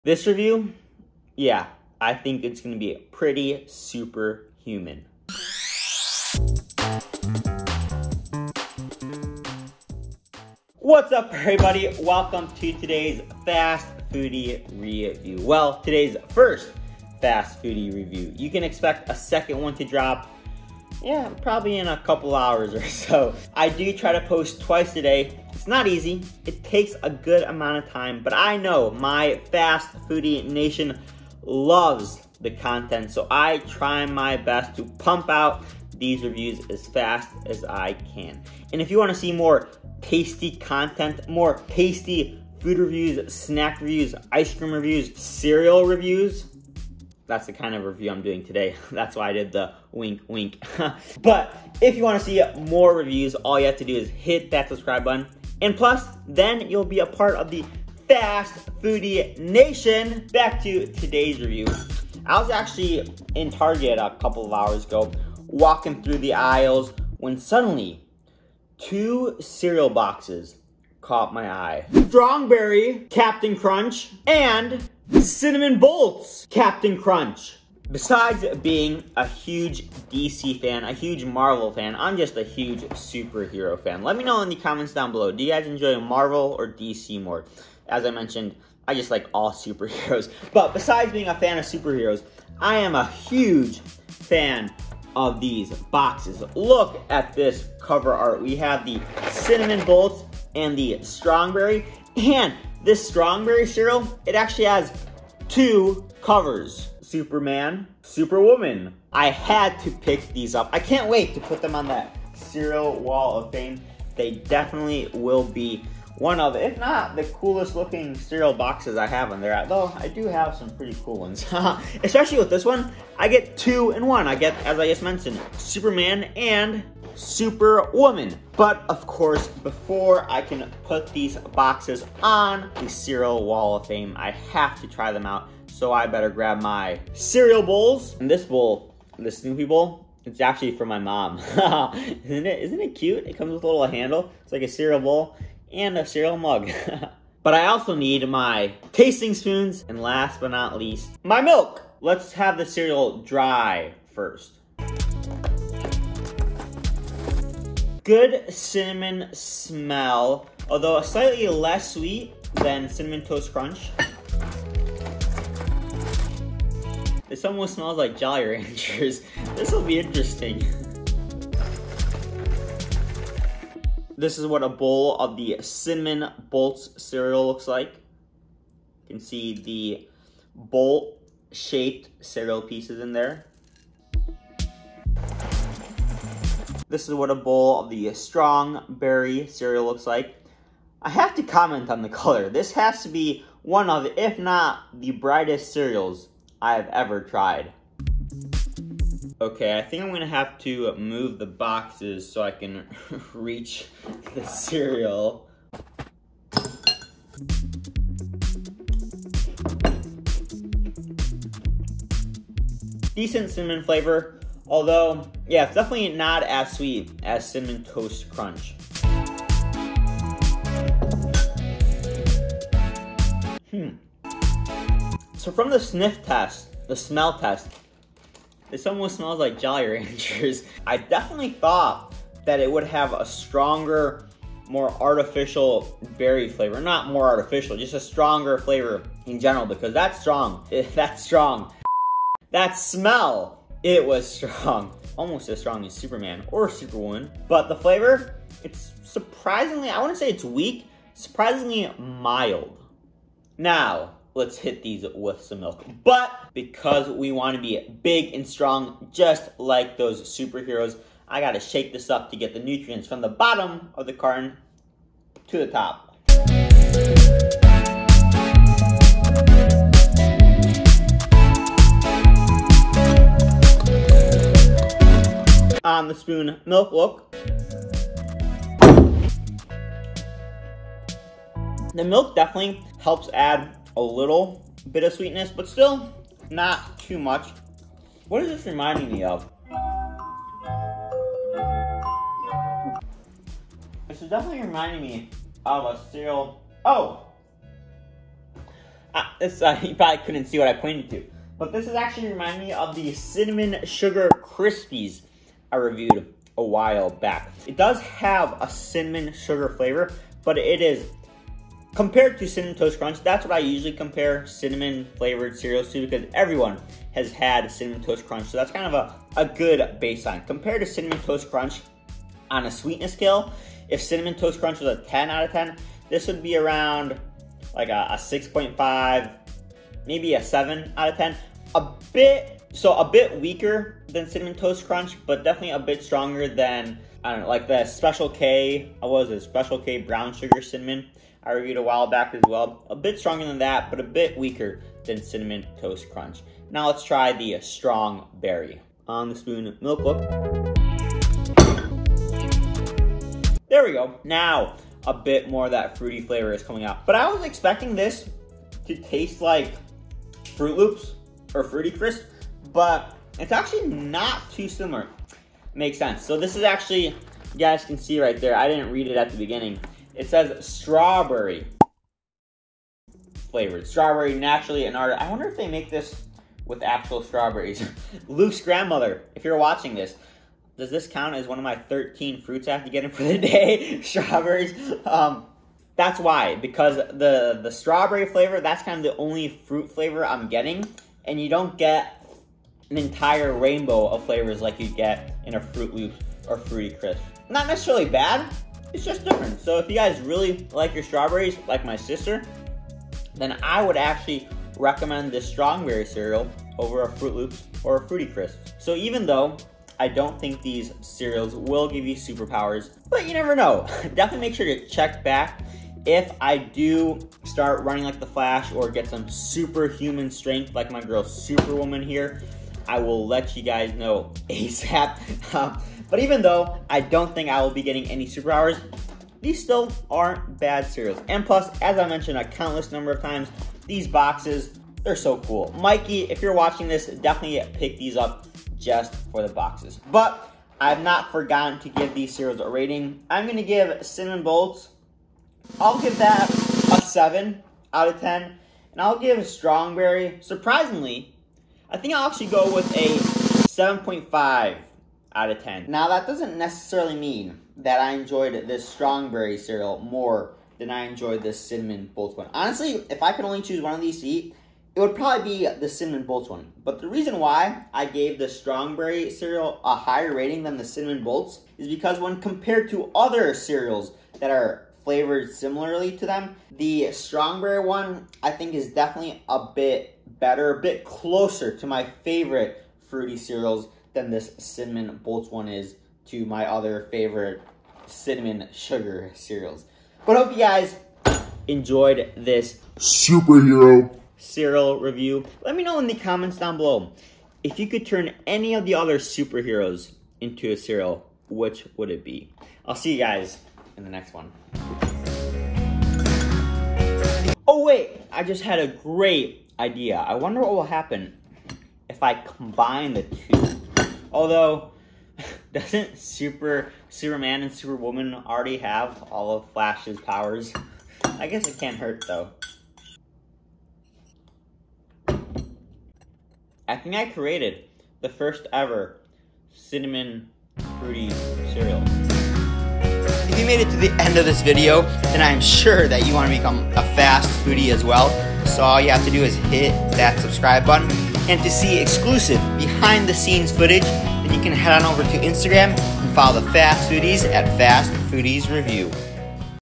DC COMICS MEETS CEREAL! New Superhero Cereal REVIEW! Cinnamon Bolts AND Strong Berry CAPN CRUNCH!
Review of the new Cinnamon Bolts and Strong Berry Cereal!